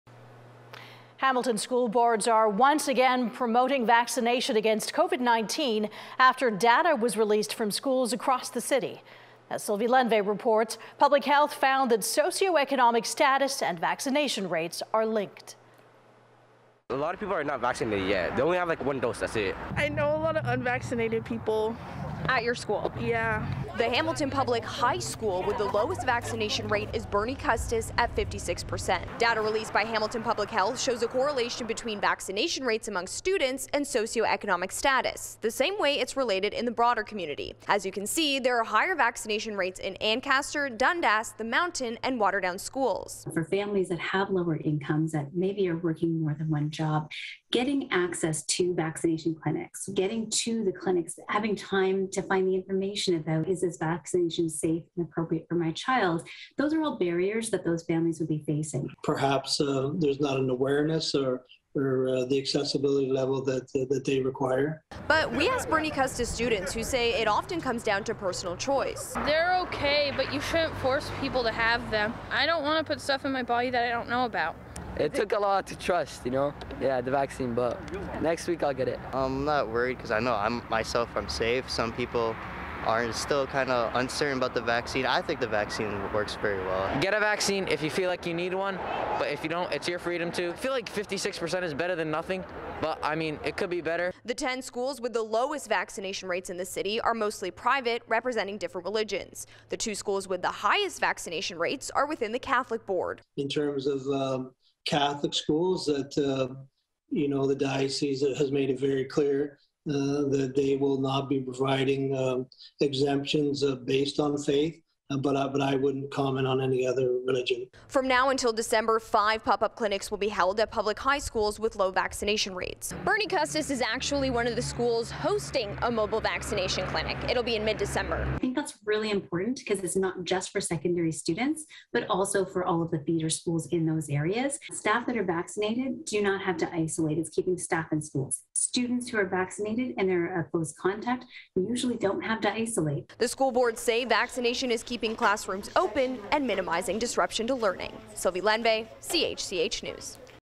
Hamilton school boards are promoting vaccination after public health releases data
Hamilton school boards are once again promoting vaccination against COVID-19, after data was released from schools across the city.

Public health found that socio-economic status and vaccination rates are linked.

Sylvie Lendvay reports.